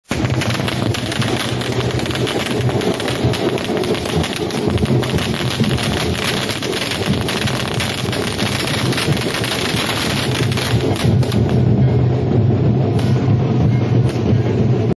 Gujarat Election Results | Video: Firecrackers Go Off As BJP Gains Huge Lead In Gujarat
Gujarat Poll Results: As the BJP sprinted ahead in 152 seats in the Gujarat Assembly elections, the party's headquarters in Ahmedabad has already begun celebrating its victory. Firecrackers were burst by supporters as they danced and hailed the party's massive win.